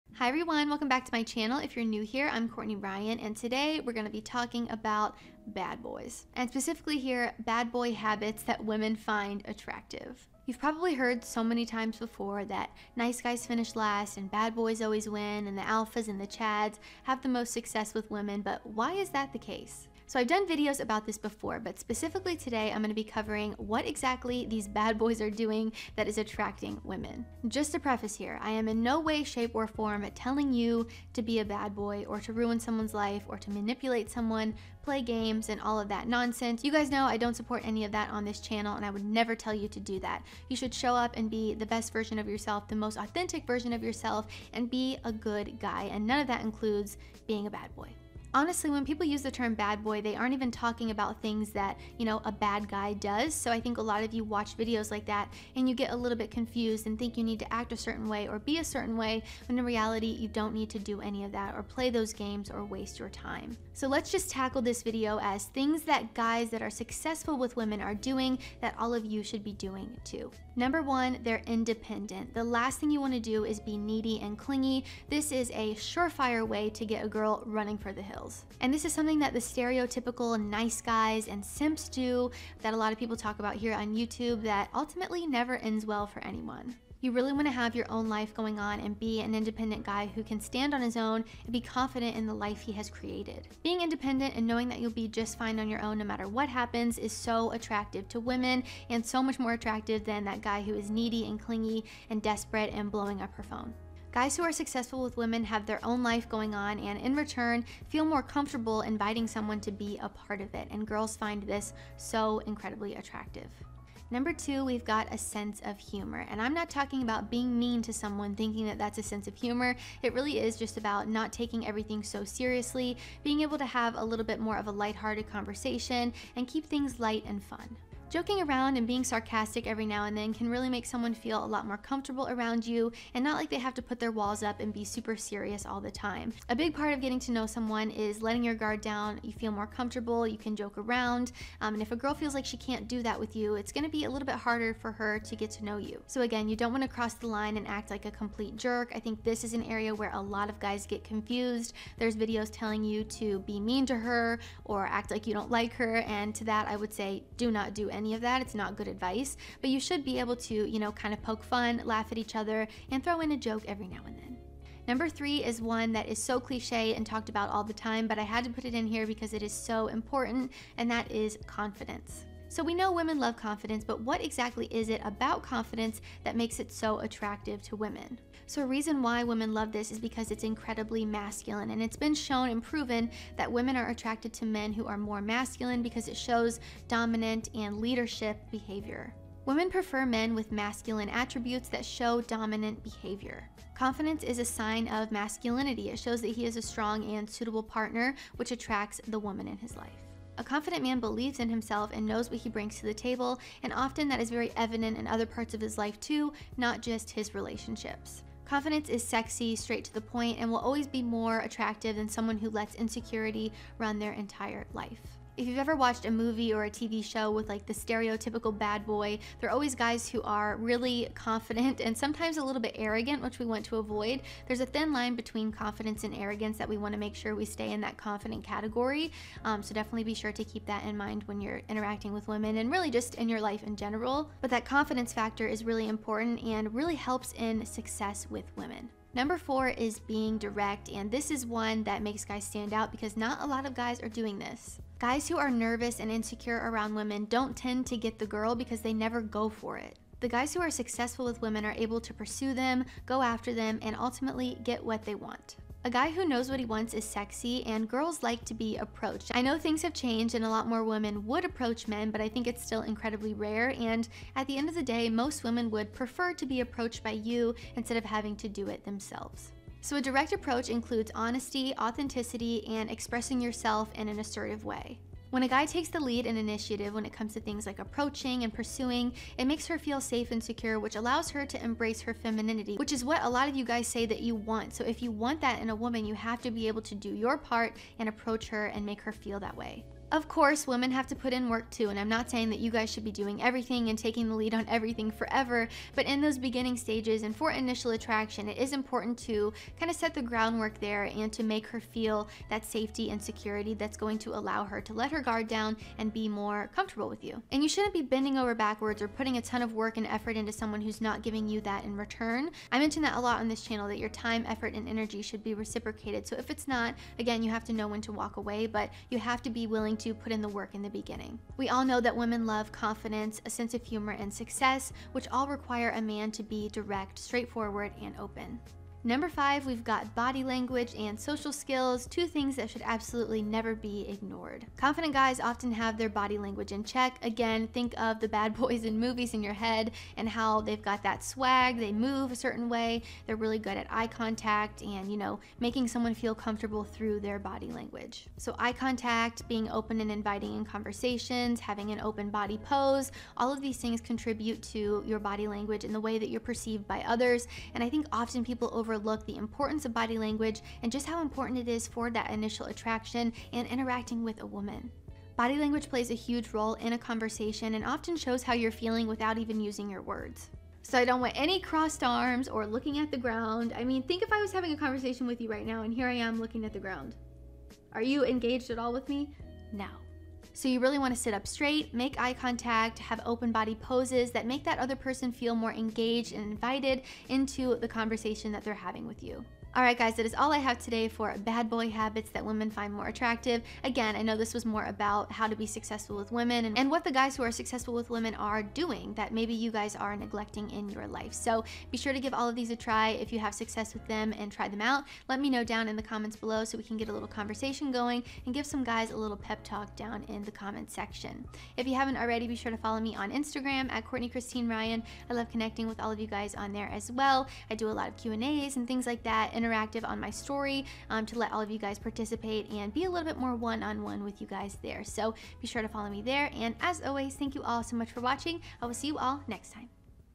"Bad Boy" Traits That Women Find Attractive | Courtney Ryan
You’ve probably heard so many times “bad boys win and nice guys always finish last” or something along those lines. I think this terminology often leaves guys feeling confused (and rightfully so) so in this video I break down the things guys do that make them successful with women. I hope this video makes things a bit more clear!